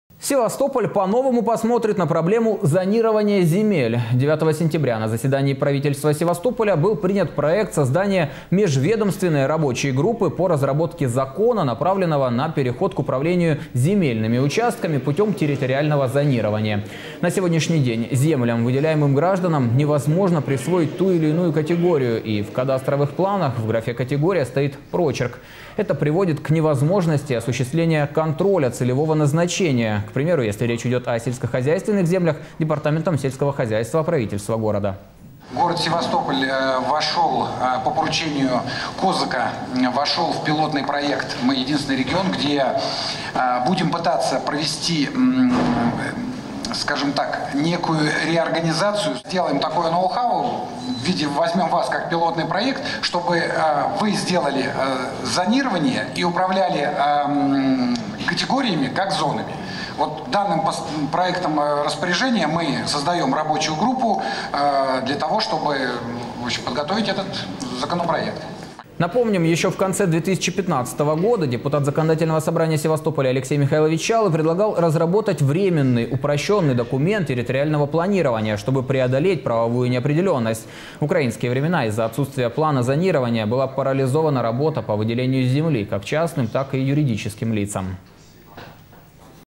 09.09.2016 Севастополь по-новому посмотрит на проблему зонирования земель
На сегодняшний день землям, выделяемым гражданам, невозможно присвоить ту или иную категорию, и в кадастровых планах в графе «категория» стоит прочерк. Это приводит к невозможности осуществления контроля целевого назначения.